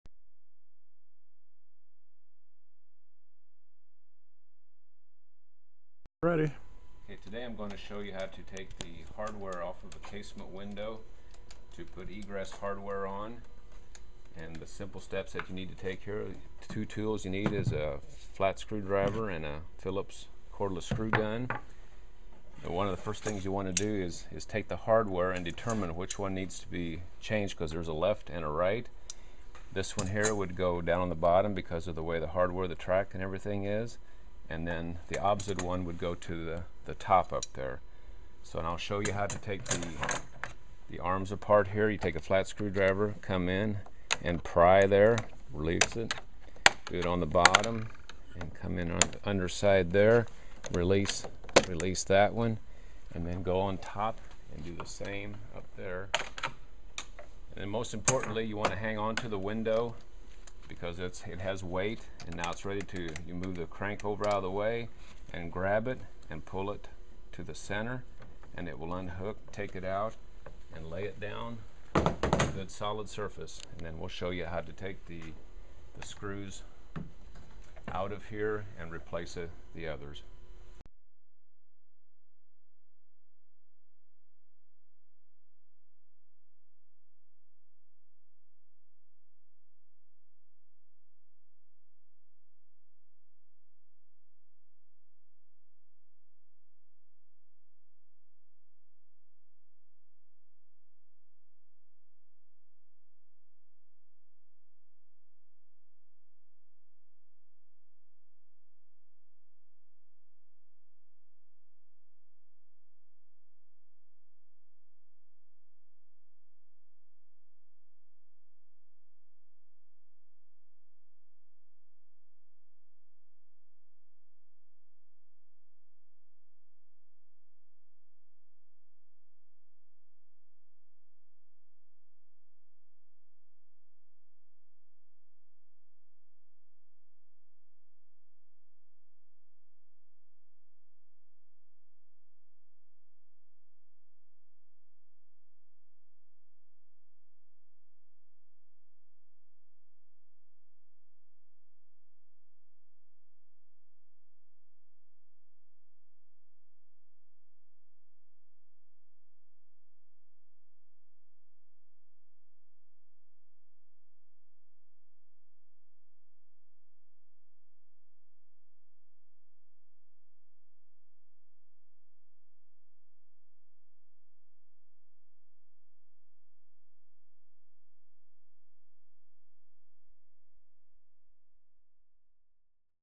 Changing Hardware on a Vinyl Window
When you need to change window hardware near the North Pole what do you do? Send them a YouTube video.